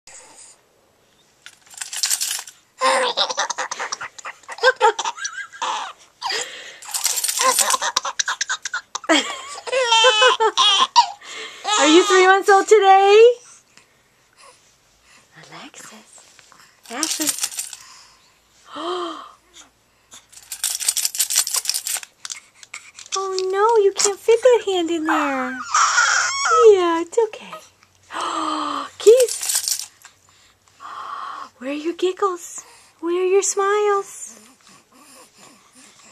Alexis giggling beside Ashley... 3 months old!